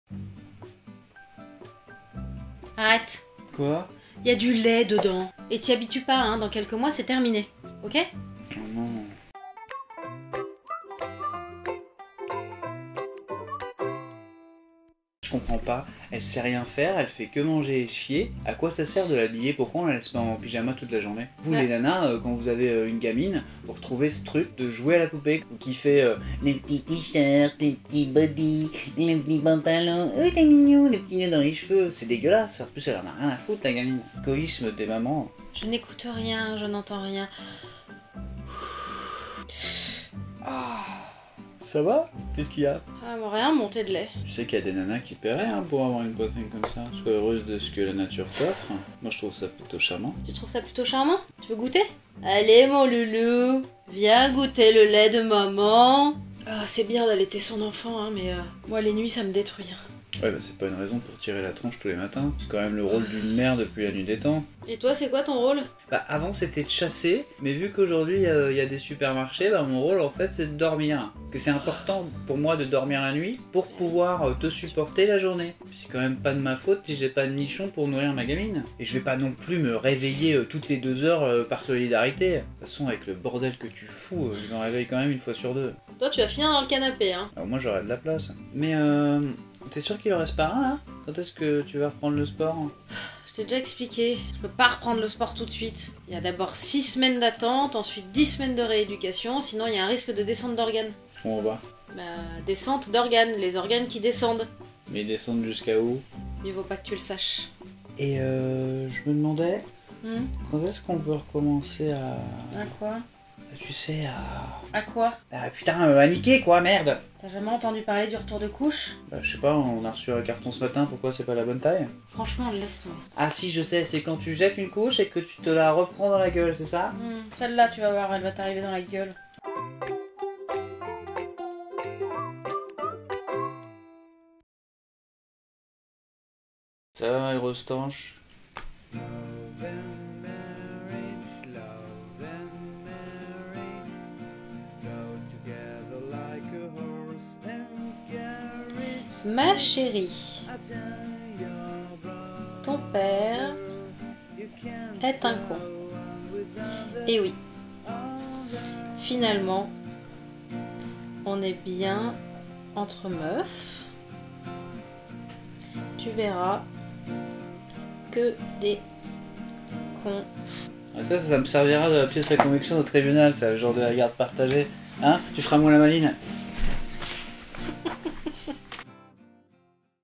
L'Allaitement !
Et si les femmes voulaient un enfant uniquement pour pouvoir à nouveau jouer à la poupée ?

Ici on parle réveil(s) nocturne(s), rééducation du périnée, reprise du sport, reprise du sexe, montée de lait, descente d'organes, retour de couches... Que des trucs cool ! ^^